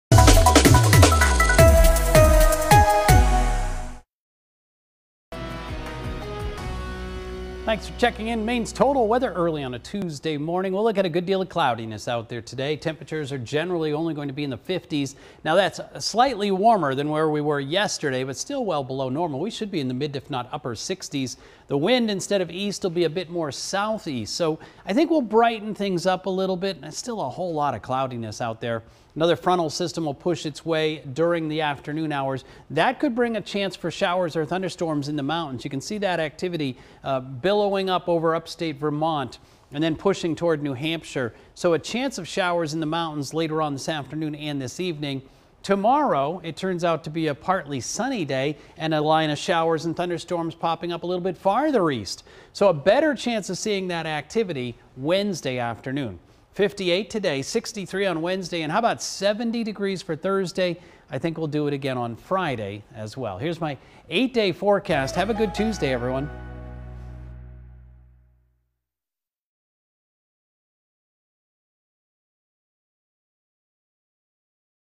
Another day of gray skies
How’s the weather looking for your Tuesday morning? Get the latest Maine’s Total Weather video forecast from Chief Meteorologist Roger Griswold.